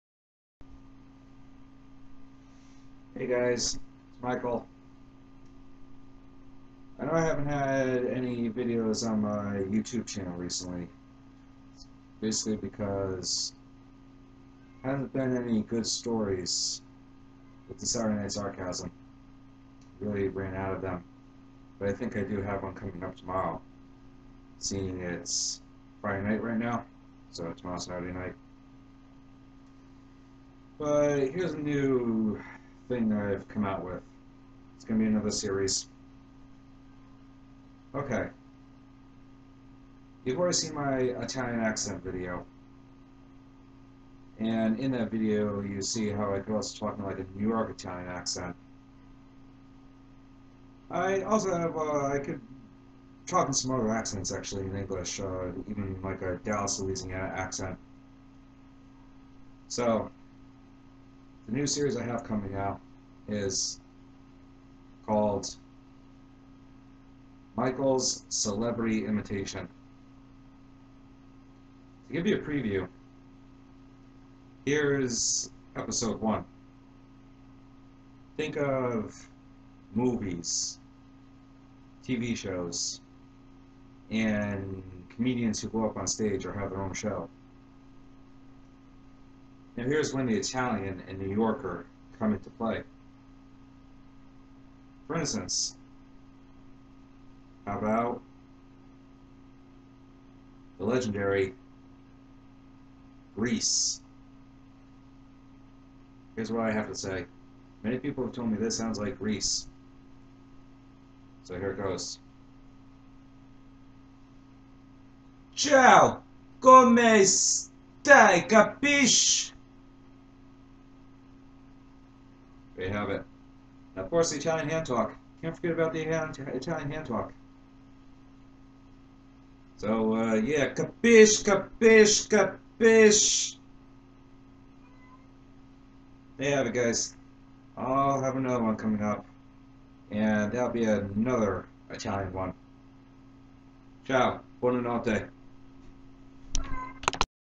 Michael's Celebrity Imitation Epsiode 1 Grease
A new series where I imitate celebrities who starred in movies, shows, and stage. Please reply telling me how well I sound.